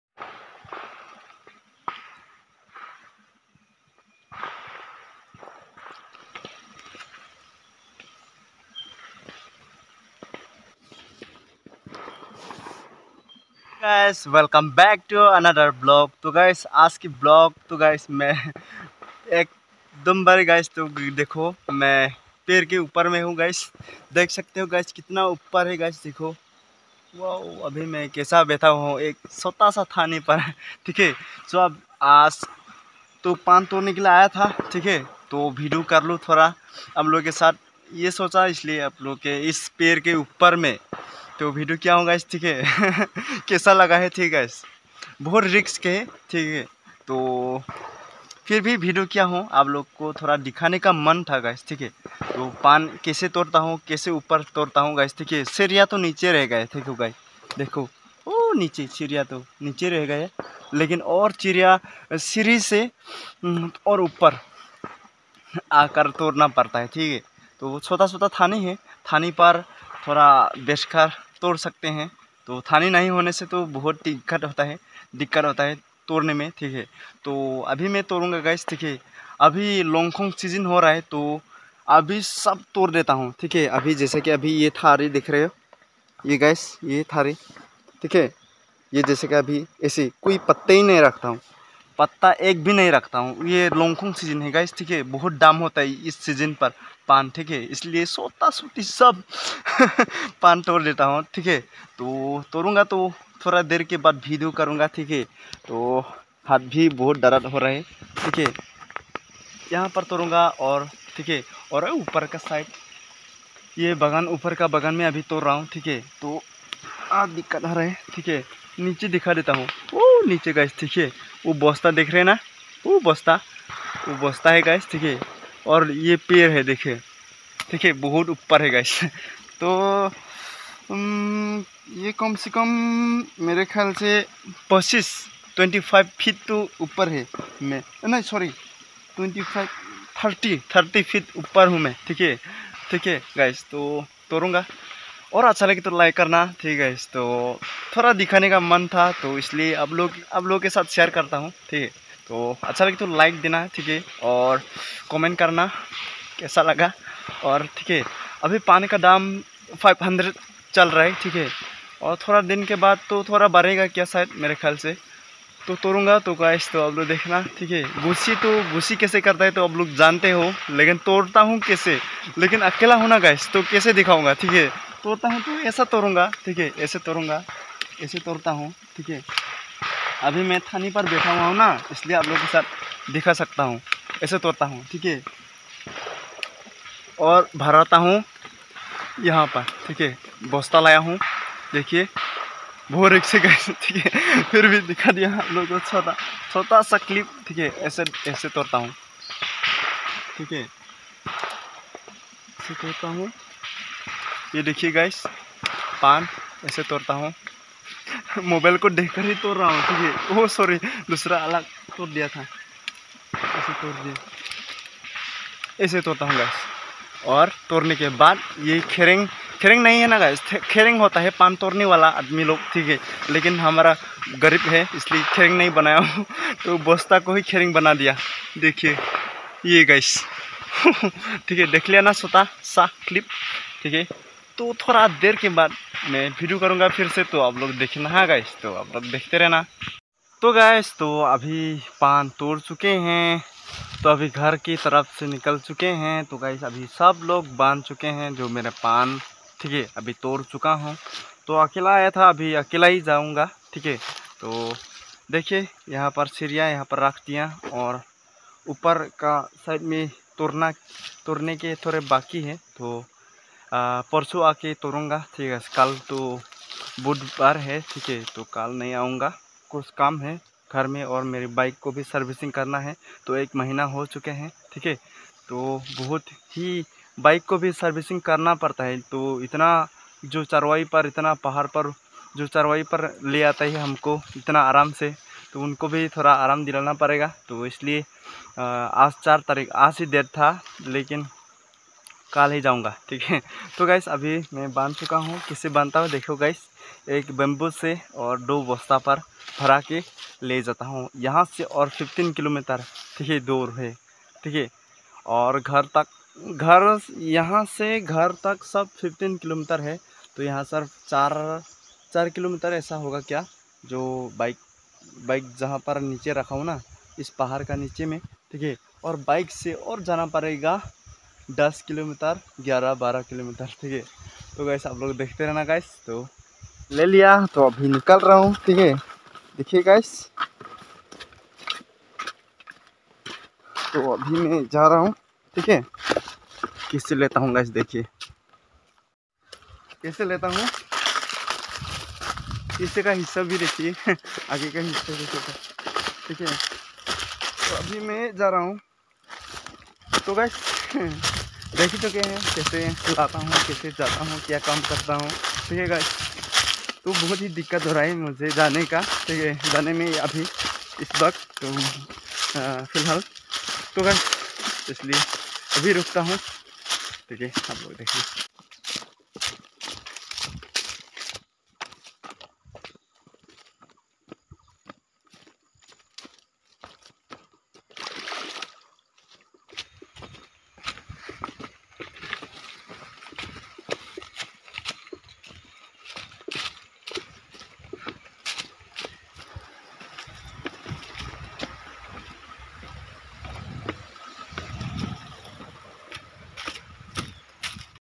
Pan Kaise Torta Hu 🌿🍃 • #219 Khasi pan, pan ki kheti, @_Prakash_Keleng_Vlog my first vlog
Pan Kaise Torta Hu Guys • Khasi pan, pan ki kheti, ‎@_Prakash_Keleng_Vlog  my first vlog

Pan ki kheti
Khasi pan
Pan Bagan